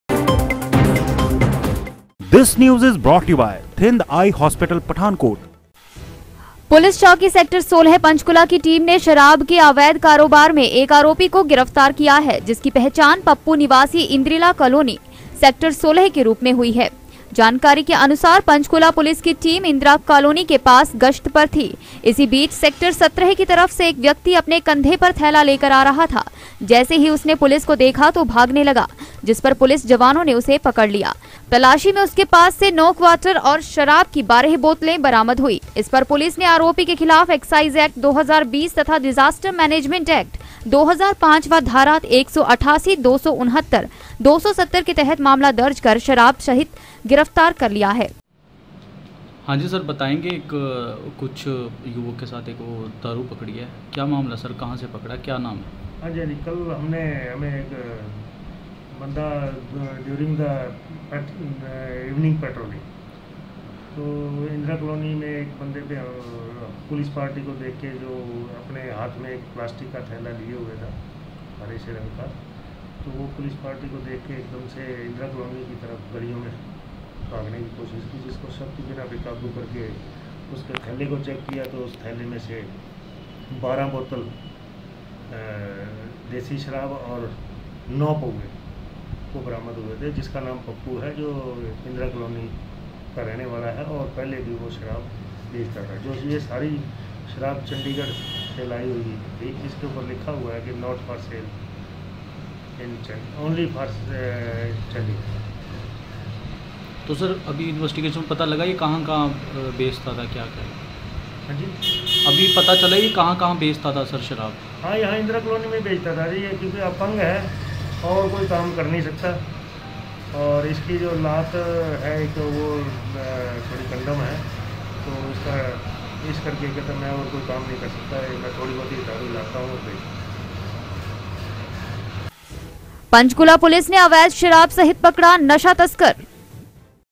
पंचकूला पुलिस ने अवैध शराब सहित पकड़ा नशा तस्कर
divyahimachaltv
dhtv,tvnews
local news
divyahimachalgoodmorning
khabre ftafat
divyahimachalgoodafternoon